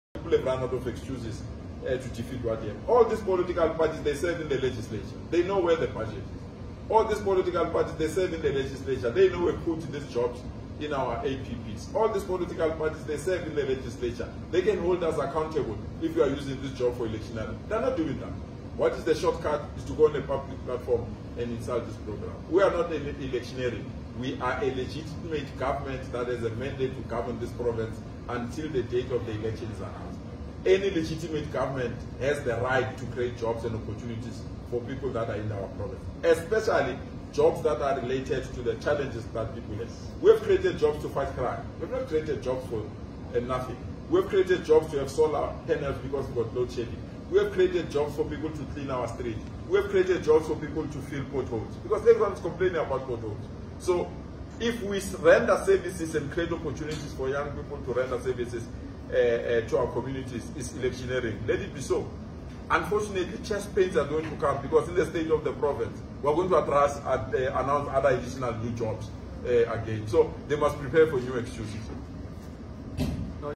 Gauteng ANC Chair Panyaza Lesufi Denies Electioneering Allegations Swiftly#ekurhuleninews
ANC Gauteng Provincial Chairperson Cde Panyaza Lesufi responding to allegations levelled against the African National Congress in Gauteng to be using the Nasi ispan campaign as an electioneering tool.